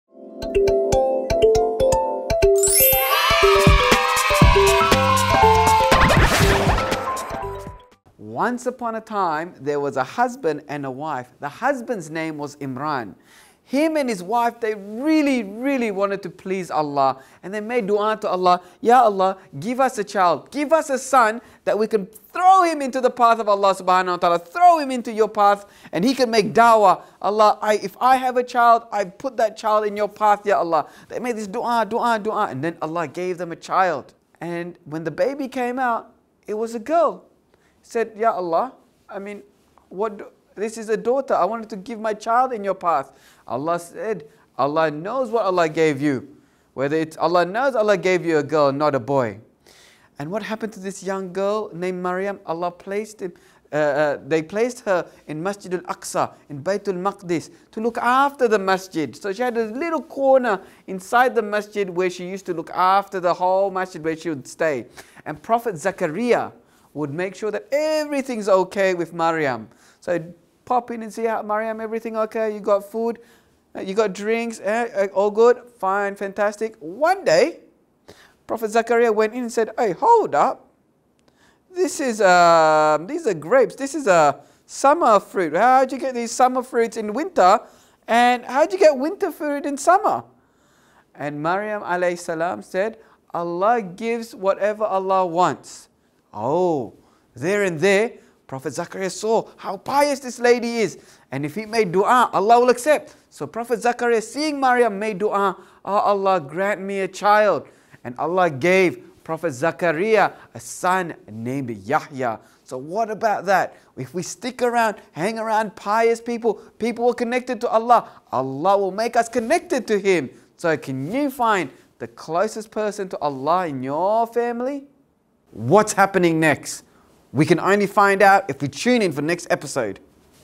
The Story of Prophet Isa - Part 1 | Imam Luqman Najib | Stories of The Prophets
The story of Prophet Isa begins with us learning about his blessed mother, Maryam. In this episode, Imam Luqman tells us some beautiful stories about Maryam, peace be upon her.